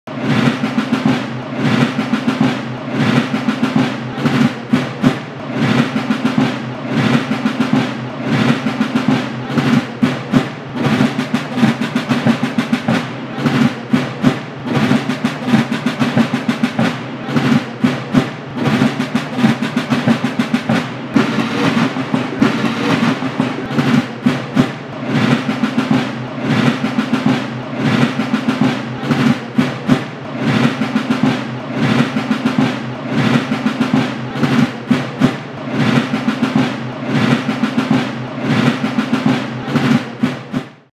Pre Palio Parade Nobile Contrada del Nicchio - Siena
Quick montage over some recordings of a pre Palio drum parade from the Contrade del Nicchio (Seashell Contrade) happening in the streets of Siena  - Via dei Rossi - in the week preceding the 2nd Palio race - the Palio dell'Assunta taking place at 16th of August - of 2021.